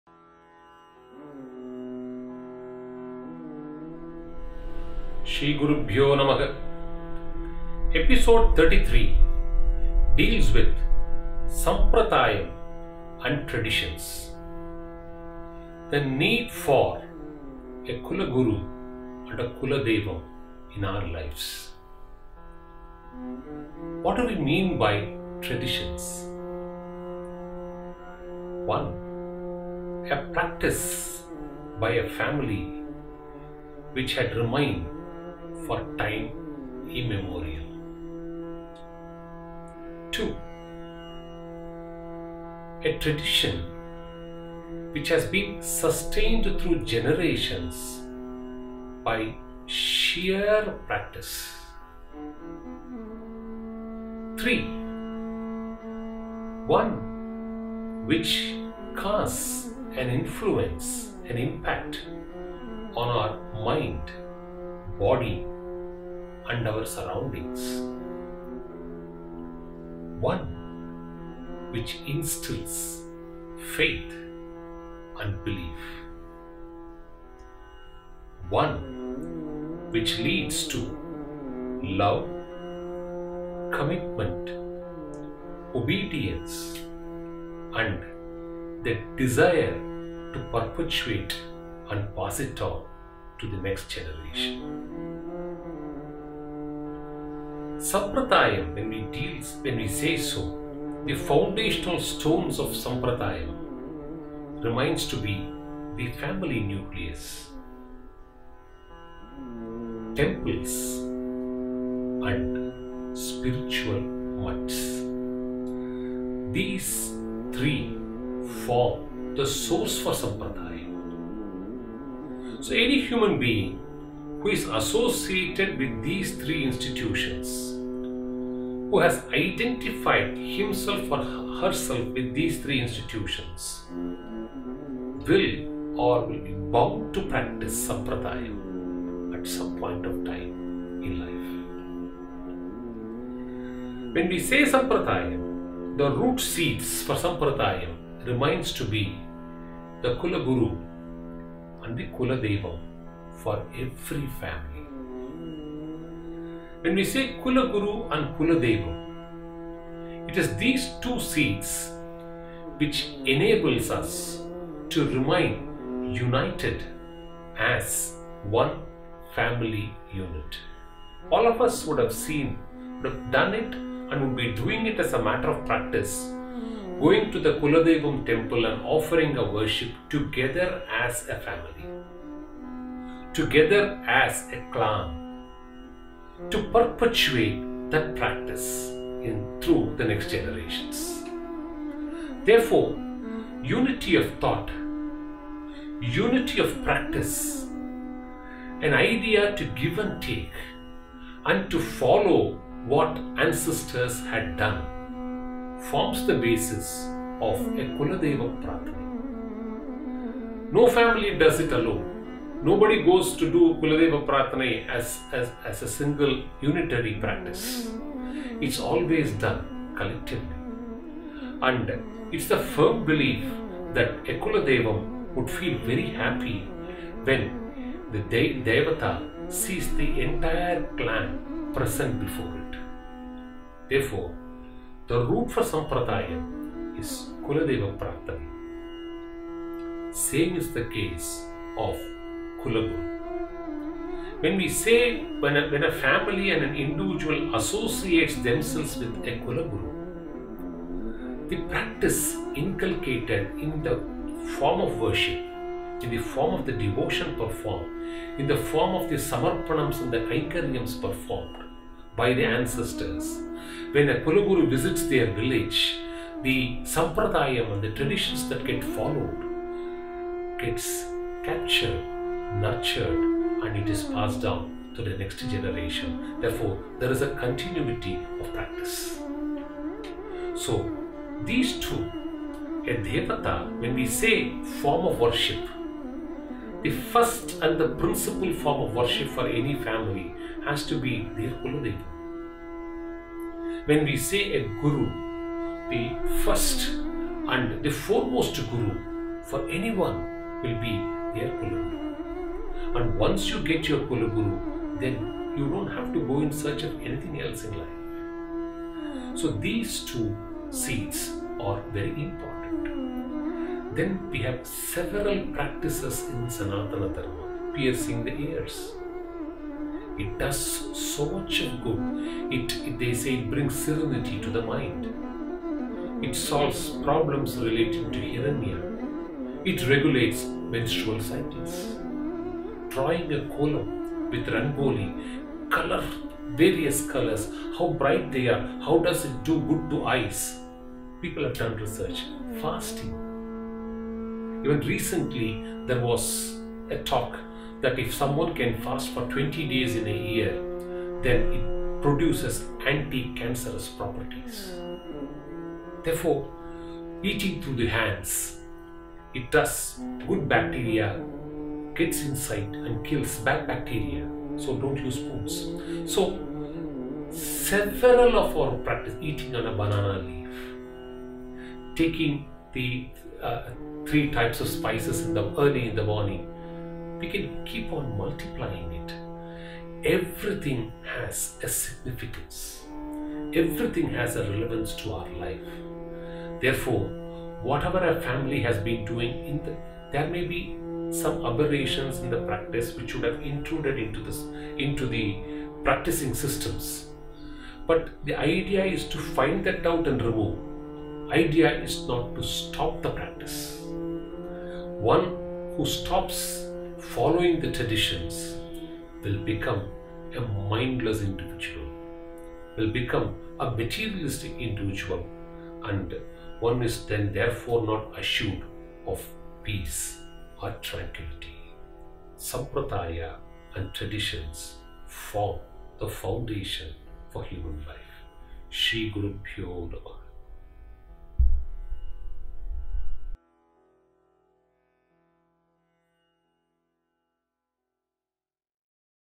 Episode 33 - Stories and Parables as narrated by Jagadguru Srimad Abhinava Vidyatirtha Mahaswamigal
SAMPRADAYAM & TRADITIONS

1. What is a Tradition? One that gets practiced, preserved and passed on to the next generation without discontinuity.

2. Why do we need Traditions?

3. What are the Foundational stones of Sampradayam?
Family, Temples and Spiritual Mutts.

4. Who are the root seeds of Sampradayam & Traditons?
Its the Kula Guru and Kuladeivam.

5. What happens to one who abandons it?

Will not one become mindless and materialistic individual, losing peace of mind.

Acharyal therefore highlights why one should practice them in Life.